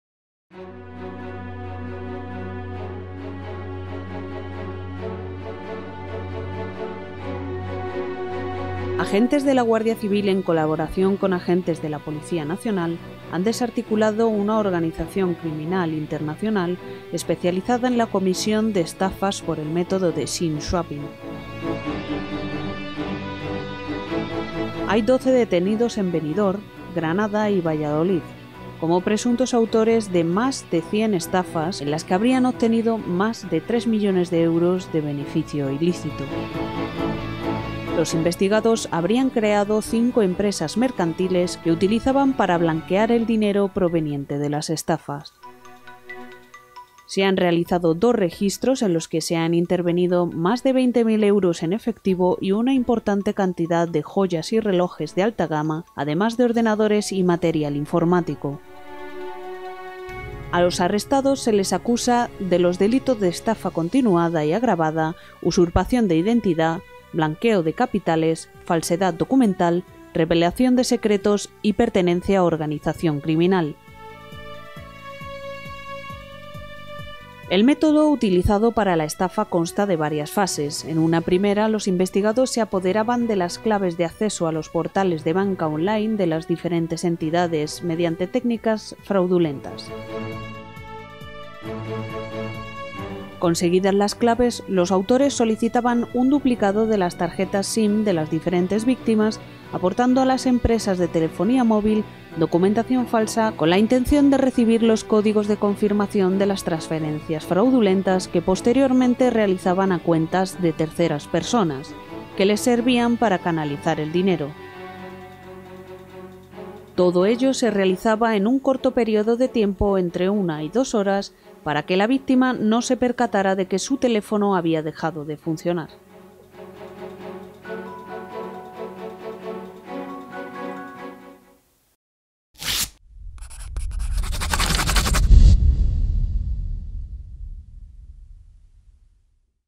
Desarticulada una organización criminal especializada en estafas por el método de SIM swapping.
Operación conjunta de la Guardia Civil y de la Policía Nacional

Desarticulada una organización criminal internacional especializada en la comisión de estafas por el método de SIM swapping

Hay doce detenidos en Benidorm (5), Granada (6) y Valladolid (1), como presuntos autores de más de 100 estafas en las que habrían obtenido más de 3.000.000 de euros de beneficio ilícito

Los investigados habían creado cinco empresas mercantiles que utilizaban para blanquear el dinero proveniente de las estafas

Se han realizado dos registros en los que se han intervenido más de 20.000 euros en efectivo y una importante cantidad de joyas y relojes de alta gama, además de ordenadores y material informático

11-marzo-2020.- Agentes de la Guardia Civil en colaboración con agentes de la Policía Nacional, han desarticulado una organización criminal internacional especializada en la comisión de estafas por el método de SIM swapping. Hay doce detenidos en Benidorm (5), Granada (6) y Valladolid (1), como presuntos autores de más de 100 estafas en las que habrían obtenido más de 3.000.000 de euros de beneficio ilícito. A los arrestados se les acusa de los delitos de estafa continuada y agravada, usurpación de identidad, blanqueo de capitales, falsedad documental, revelación de secretos y pertenencia a organización criminal. Los investigados habían creado cinco empresas mercantiles que utilizaban para blanquear el dinero proveniente de las estafas. Se han realizado dos registros en los que se han intervenido más de 20.000 euros en efectivo y una importante cantidad de joyas y relojes de alta gama, además de ordenadores y material informático.

La investigación, que se inició hace aproximadamente un año, ha contado con la colaboración de analistas de Europol y de la Policía Nacional de Italia.

Modus Operandi: el SIM swapping o clonado de tarjeta SIM

El método utilizado para la estafa consta de varias fases; en una primera fase, los investigados se apoderaban de las claves de acceso a los portales de banca online de las diferentes entidades mediante técnicas de “phishing”, “malware” o “pharming”. Conseguidas las claves, los autores solicitaban un duplicado de las tarjetas SIM de las diferentes víctimas, aportando a las empresas de telefonía móvil documentación falsa -en algunos casos, incluso de personajes públicos- con intención de recibir los códigos de confirmación de las transferencias fraudulentas que posteriormente realizaban.

Obtenidos esos códigos, realizaban las transferencias fraudulentas desde las cuentas de las víctimas a cuentas de terceras personas, que les servían para canalizar el dinero. En otras ocasiones, también solicitaban préstamos preconcedidos o microcréditos a las entidades bancarias, con el fin de obtener mayor beneficio económico.

Todo ello se realizaba en un corto periodo de tiempo, entre una y dos horas; tiempo máximo en el que la víctima se percataba de que su teléfono había dejado de funcionar -ya que su tarjeta SIM estaba inactiva- debido a que ya se estaba operando con la nueva tarjeta duplicada.

De esta manera, a cada perjudicado le sustraían de sus cuentas bancarias cantidades que iban desde los 6.000 hasta los 137.000 euros, en uno de los casos.

La organización criminal contaba, entre sus miembros, con nacionales de Italia, Rumanía, Colombia y España. Si bien los cabecillas de la misma eran de origen napolitano y se habían asentado, desde principios del año pasado, en la localidad de Benidorm manteniendo un contacto muy estrecho con un grupo mafioso de su localidad natal.

Blanqueo de las ganancias ilícitas obtenidas

Con el fin de blanquear las ganancias ilícitas obtenidas con las estafas, los miembros de la organización habían creado un total de cinco mercantiles con varias cuentas bancarias cada una. Todas ellas presentaban el mismo objeto social y domicilio, pero sin ningún tipo de actividad mercantil.

Dichas cuentas se nutrían de los ingresos en efectivo realizados por los miembros del grupo y, presuntamente, provenientes de las estafas cometidas. Posteriormente, con ese dinero pagaban las nóminas a los empleados de las mencionadas empresas -ocho en total- que habrían llegado a España desde Italia, donde eran dados de alta como trabajadores en la Seguridad Social. De esta forma, abrían varias cuentas bancarias y luego regresaban de nuevo a Italia, lugar donde presuntamente terminaba el recorrido del dinero estafado. Eran los propios cabecillas de la organización los que se encargaban de ingresar el dinero en efectivo para luego operar directamente con las cuentas de los empleados retirando el dinero de forma presencial, o bien transfiriéndolo directamente a cuentas bancarias italianas.